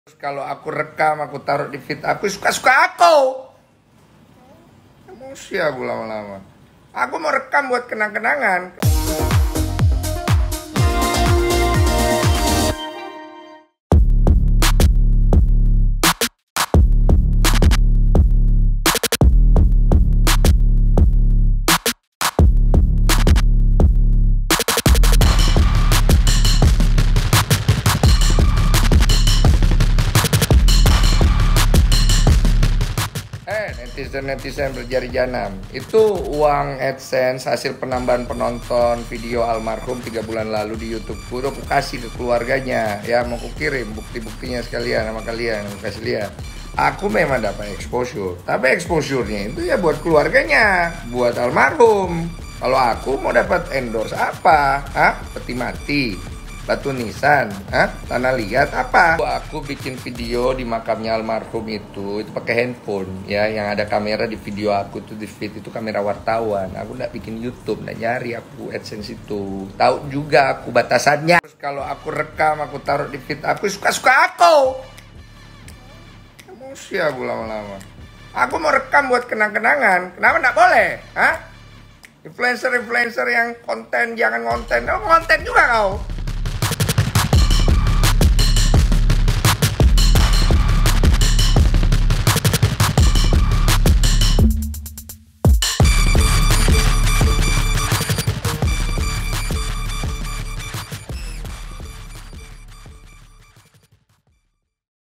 Denny Sumargo Kesal Dituding Netizen Soal Vanessa: Suka-Suka Aku
KAPANLAGI.COM - Denny Sumargo kesal dituding netizen memanfaatkan mendiang Vanessa Angel dan Bibi Andriansyah sebagai konten. Densu mengunggah video ketika dirinya sedang berziarah ke makam mereka. Ini penjelasan pebasket sombong.